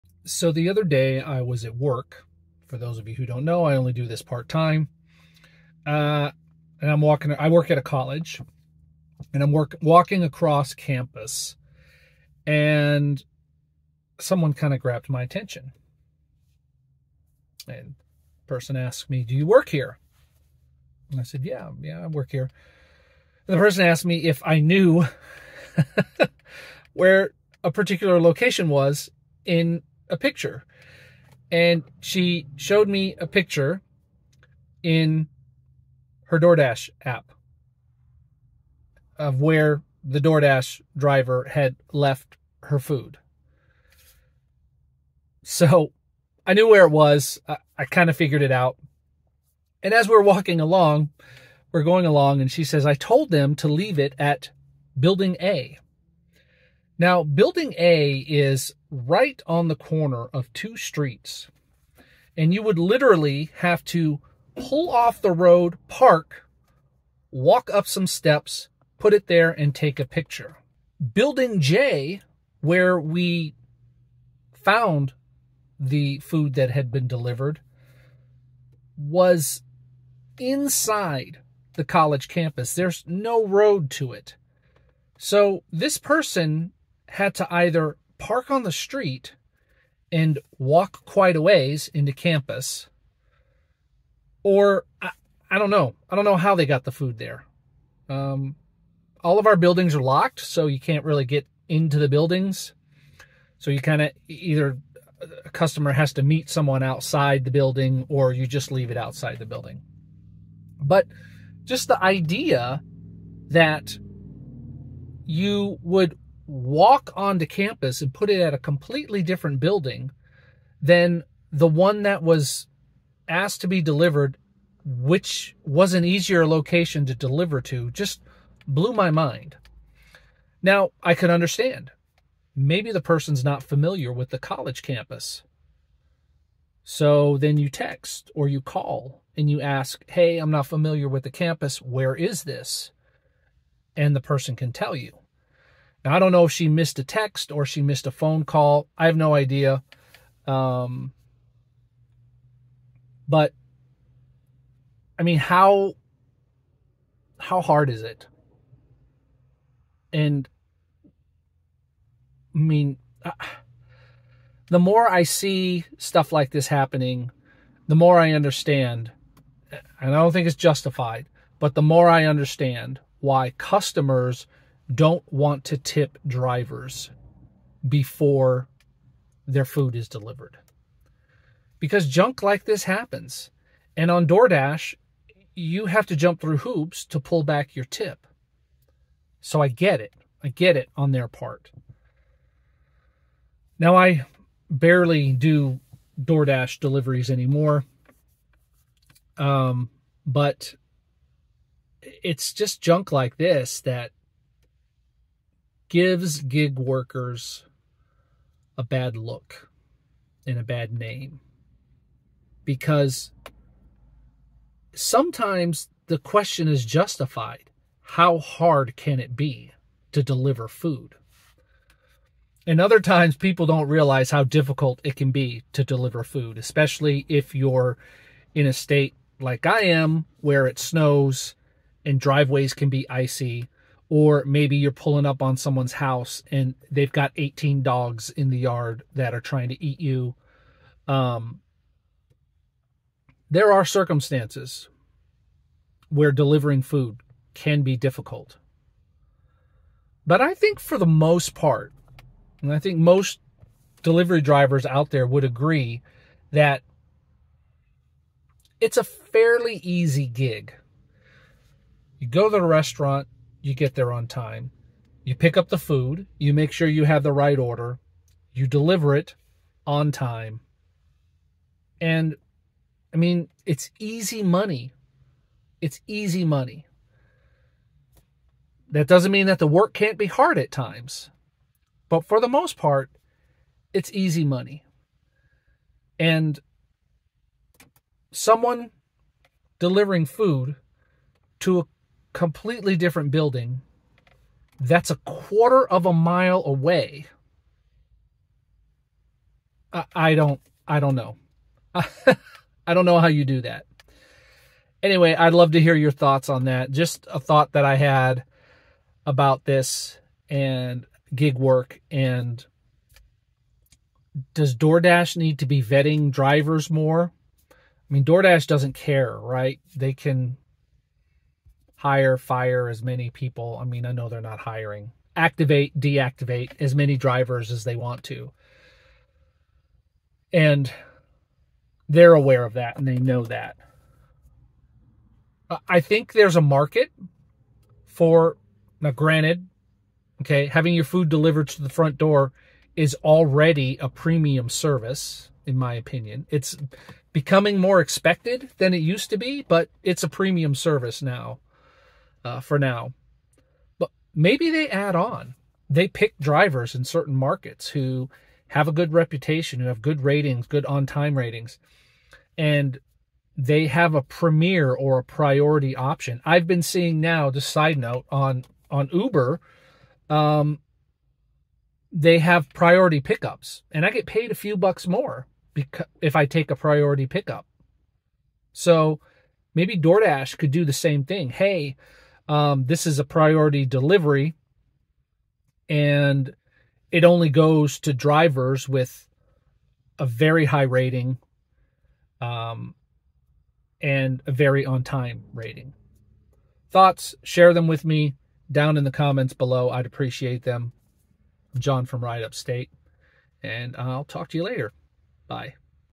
How Hard Is It to Deliver Food for Doordash?
Is it really that hard to deliver food for Doordash?

I use Gridwise to track my miles and earnings: https